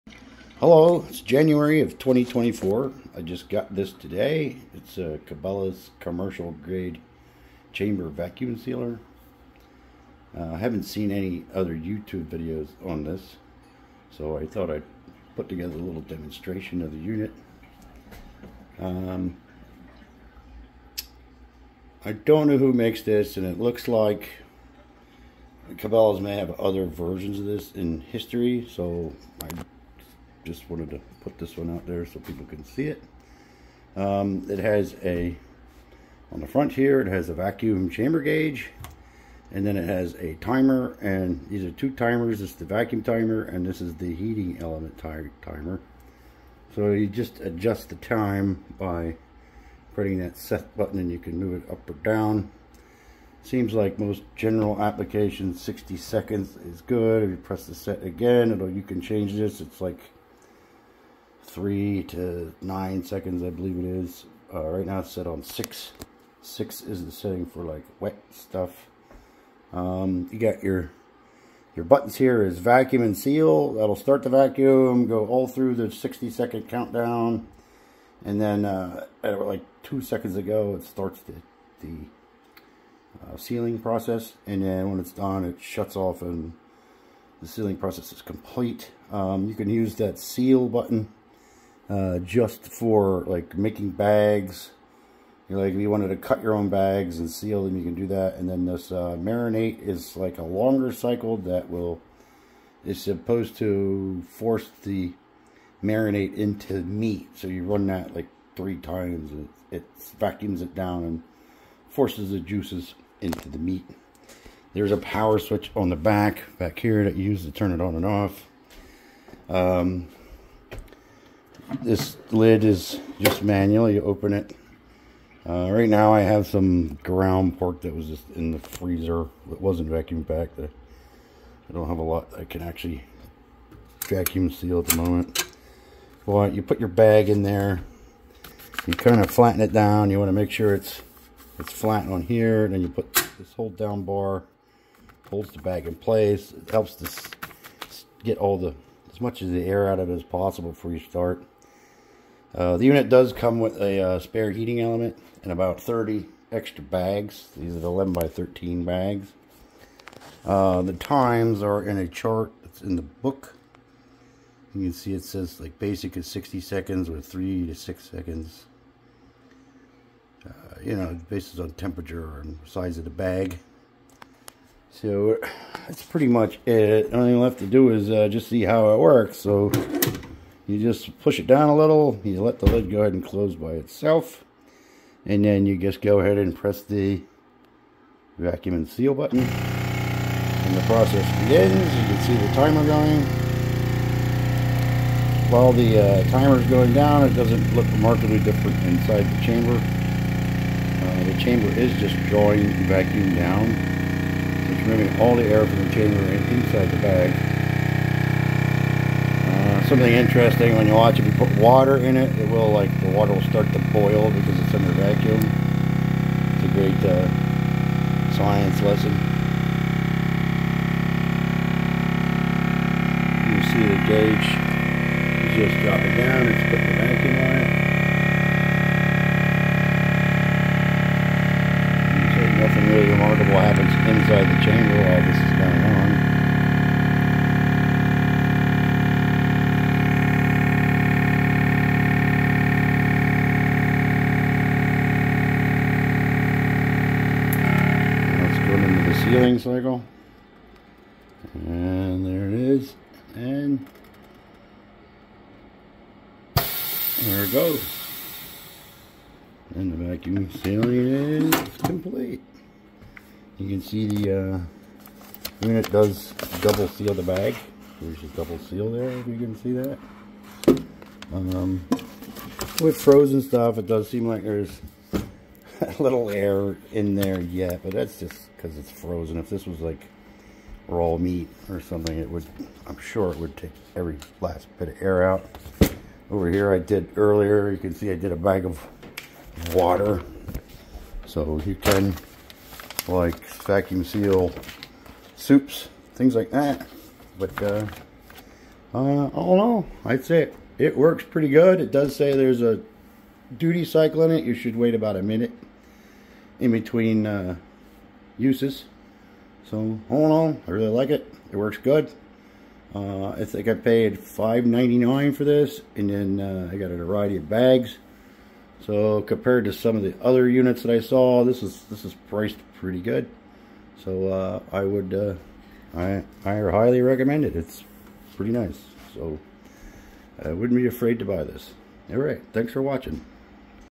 Cabela's Commercial Grade Chamber Vacuum Sealer Review & Demonstration. Purchased 1/2024.
Review & demonstration of the Cabela's Chamber Vacuum Sealer. Unit purchased 1/2024. Purchase price $599.